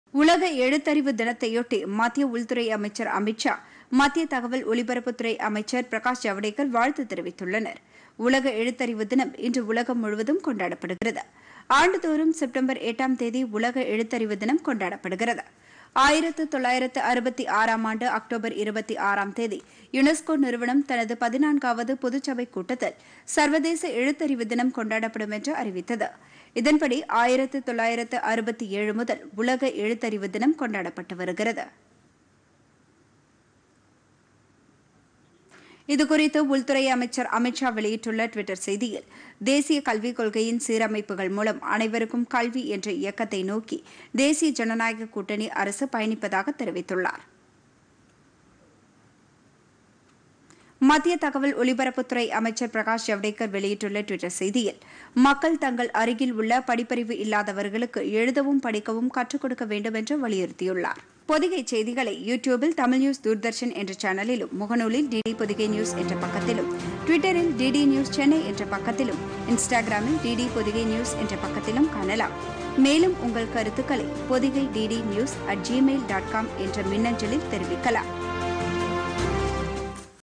உலக எழுத்தறிவு தினம்-தலைவர்கள் வாழ்த்து
உலக எழுத்தறிவு தினம் இன்று கடைபிடிக்கப்படுகிறது - மத்திய உள்துறை அமைச்சர் அமித் ஷா, மத்திய தகவல் ஒலிபரப்புத்துறை அமைச்சர் பிரகாஷ் ஜவடேகர் வாழ்த்து